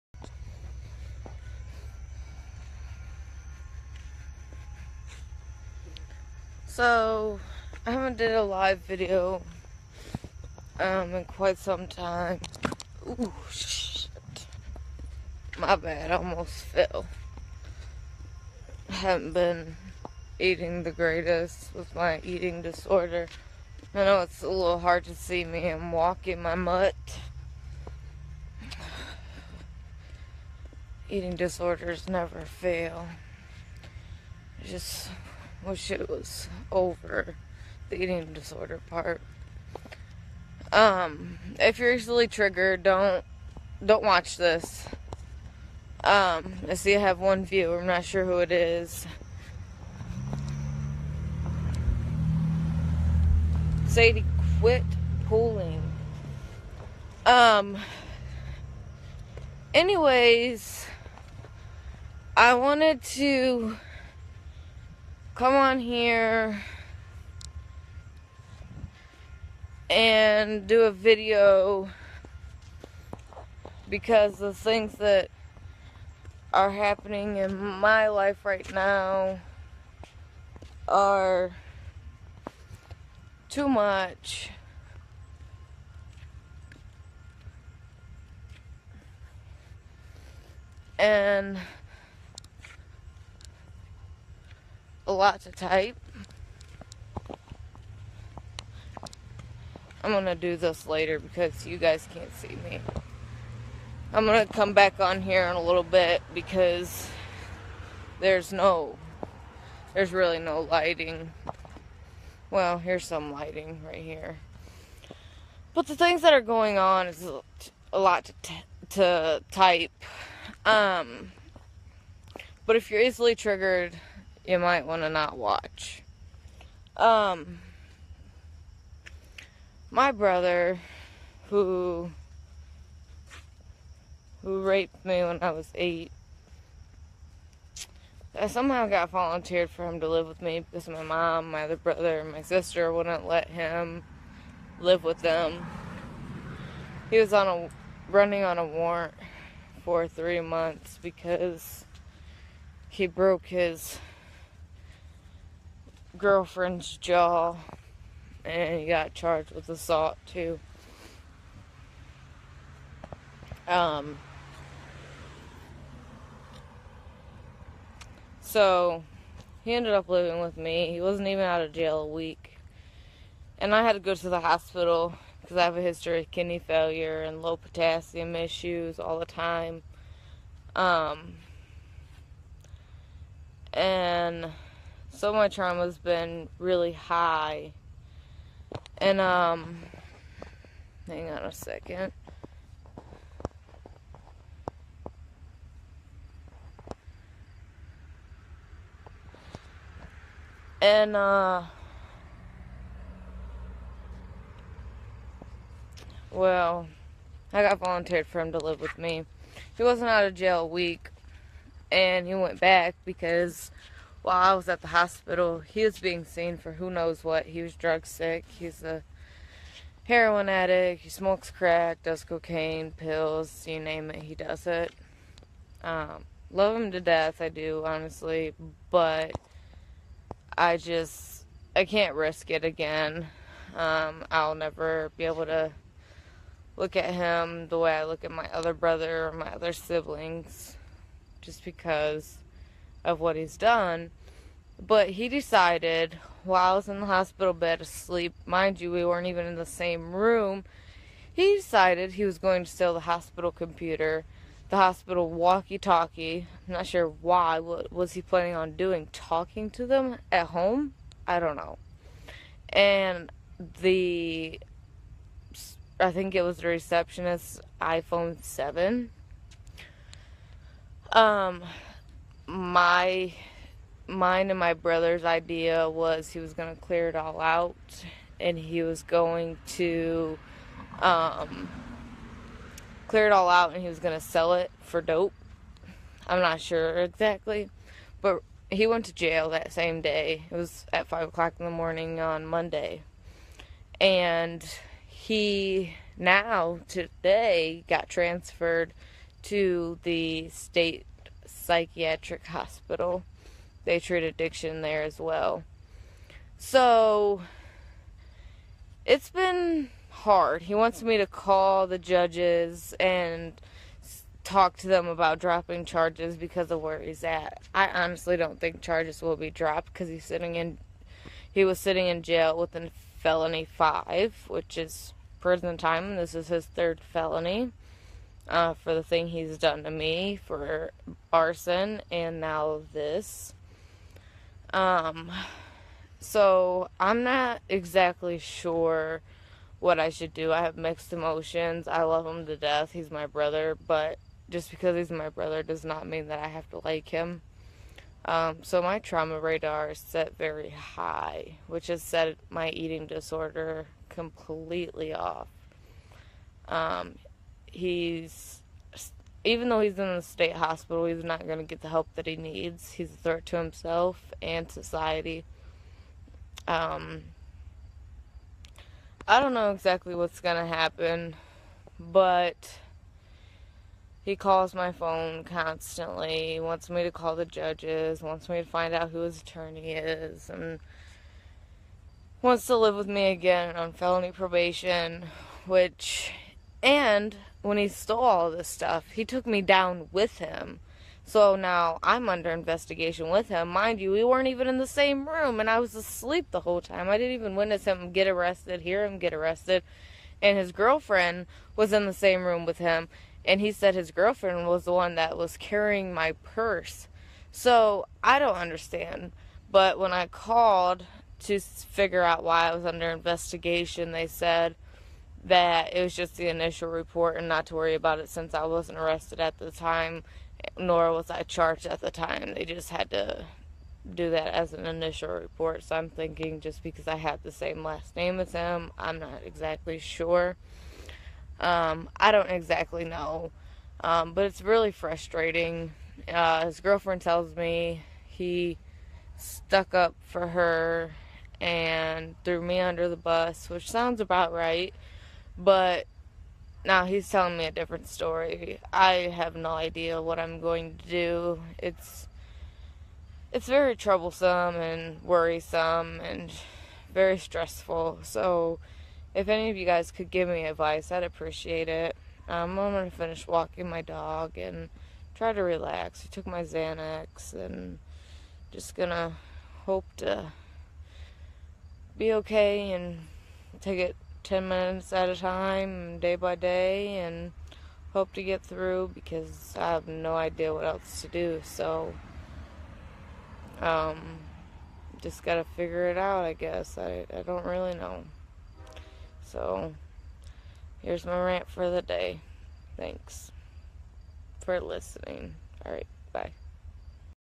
Brittany's PTSD Story
PTSD Buddies member Brittany does a live video and talks to the members of the group about her PTSD.  Thanks for sharing you are so strong.